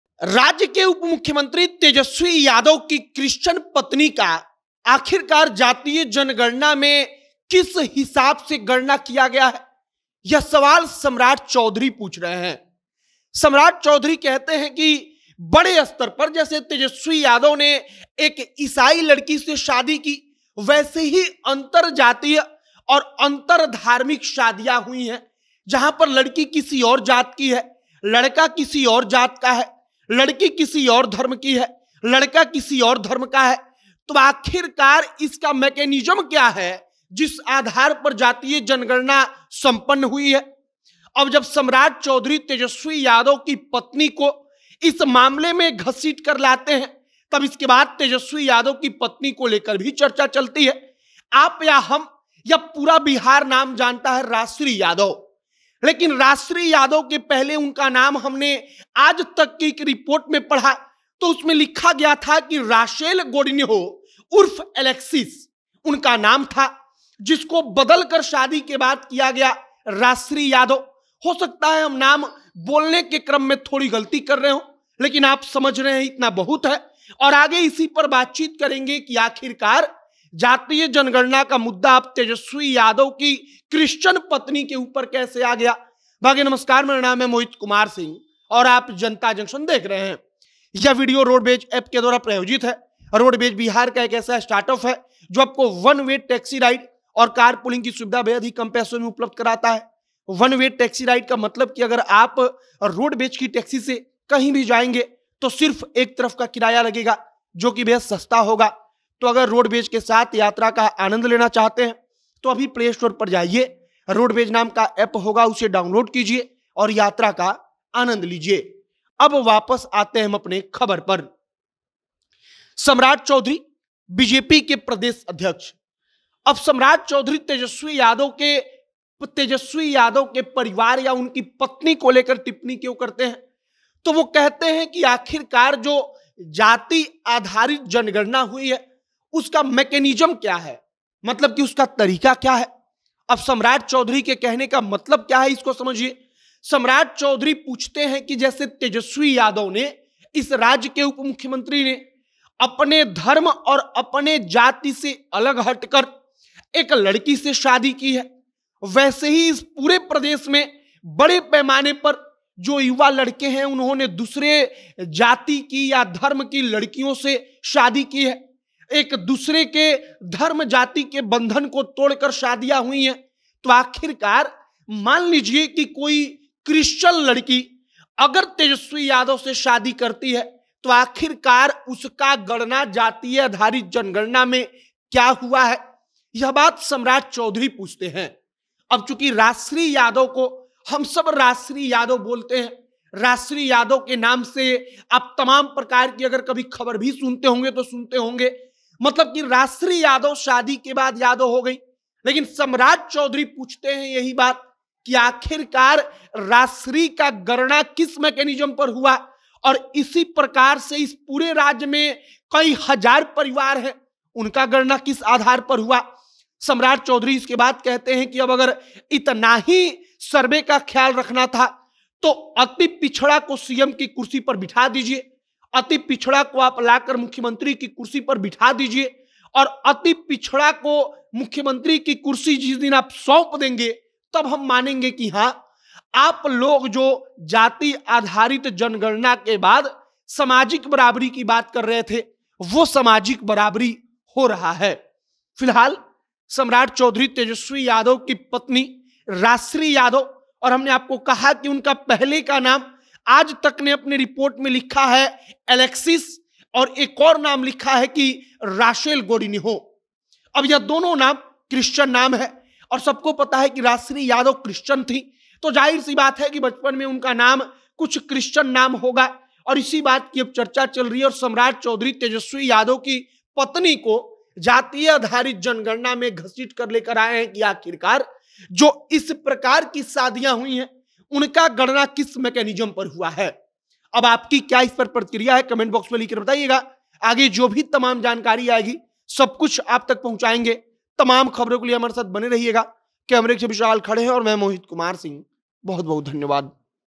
Tejashwi की पत्नी को जातीय जनगणना में Yadav लिखा गया या Christan? Samrat
तेजस्वी की पत्नी को जातीय जनगणना में यादव लिखा गया है या इसाई? सम्राट चौधरी ने तेजस्वी की पत्नी को घसीट लाया जातीय जनगणना में।